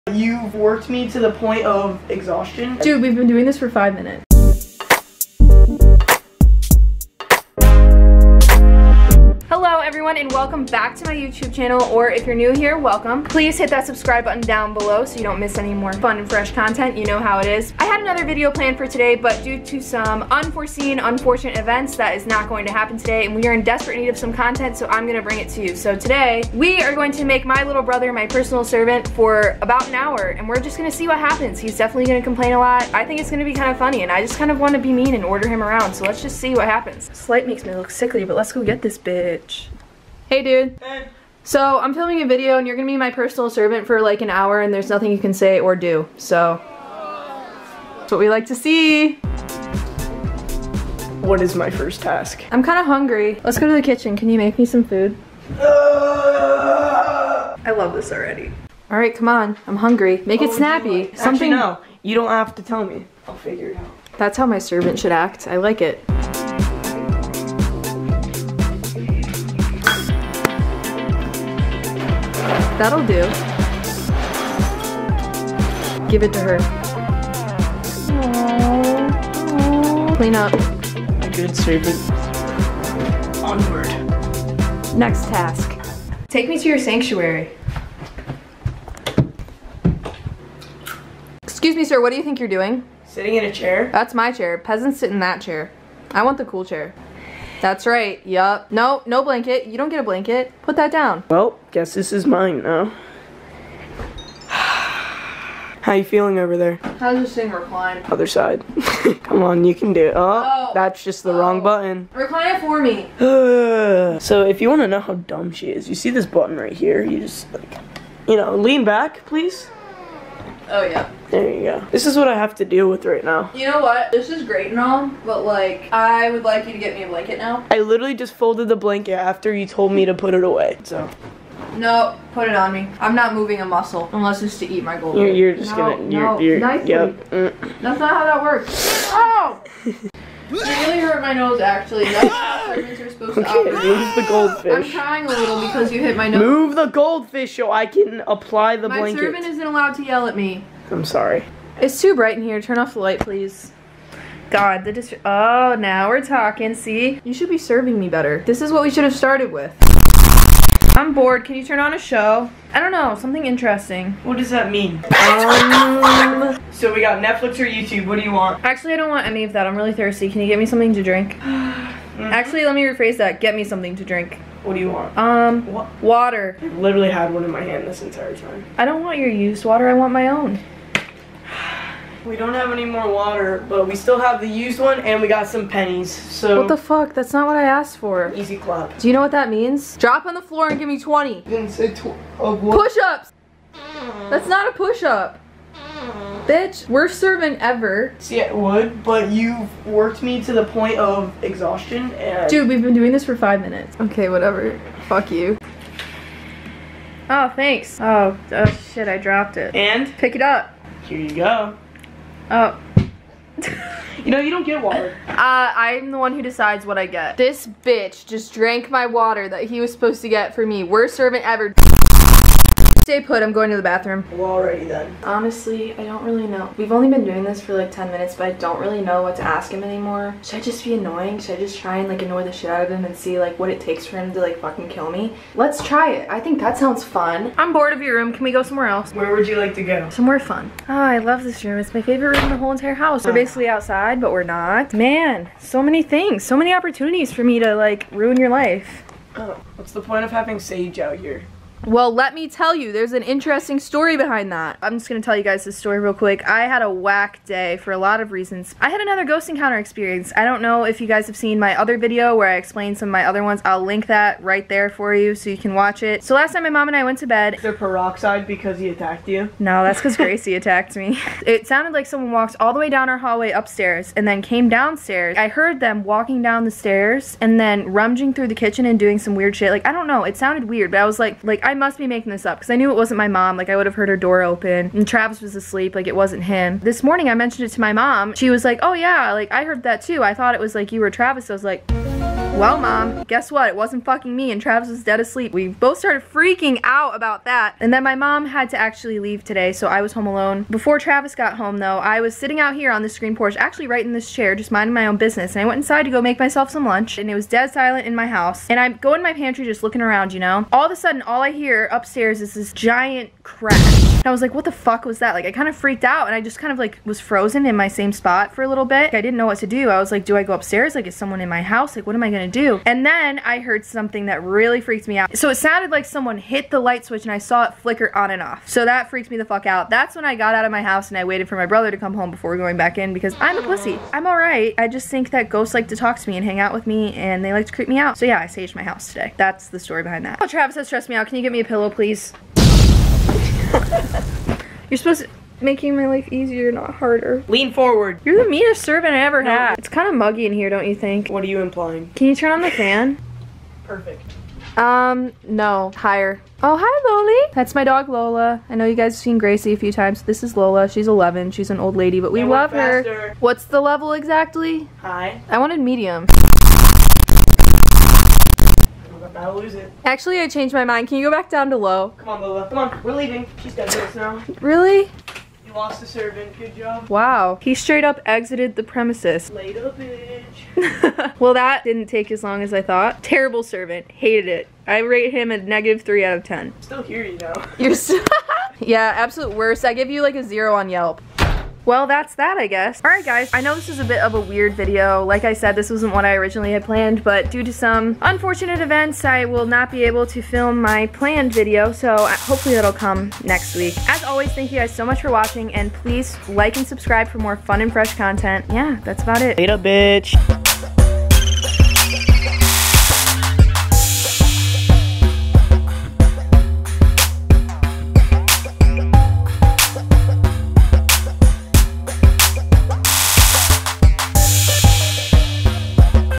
Forcing my brother to be my servant for 1 hour
I forced my little brother to do whatever I ask as my servant for 1 hour. A little blood, sweat, and tears... He hated every second of it.